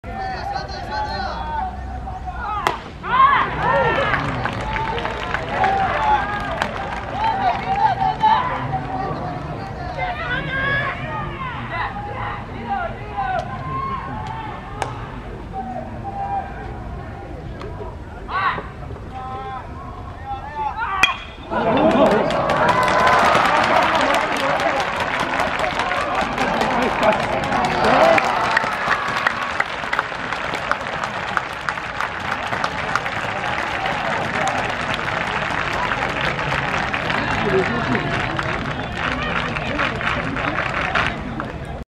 東海大学菅生高校 本橋空南太選手 左前安打
2021年7月16日 第103回全国高等学校野球選手権大会 西東京大会